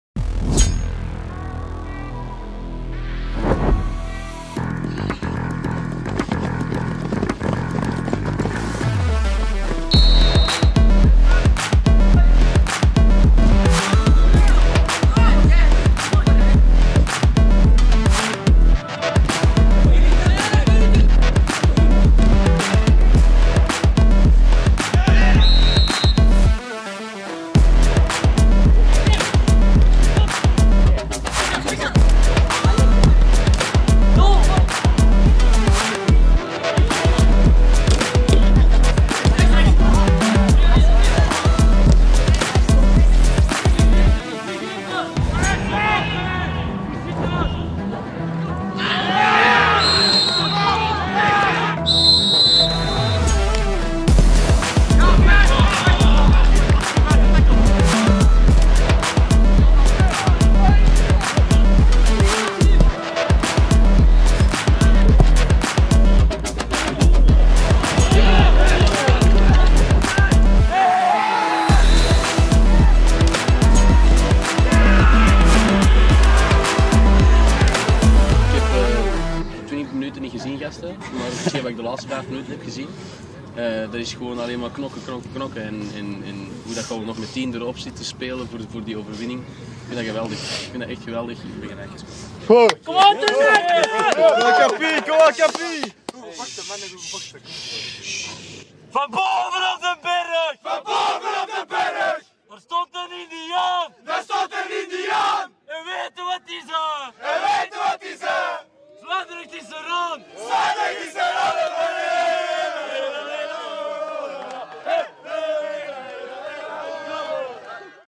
JDWSCH - Verbr. Zwijndrecht B - K. Ternesse V.V. B
A small recap of the latest game of K Ternesse VV B

Ternesse went to Verbroedering Zwijndrecht to try to take the 3 points home. It was a tough game without a winner. As result a 3-3 draw.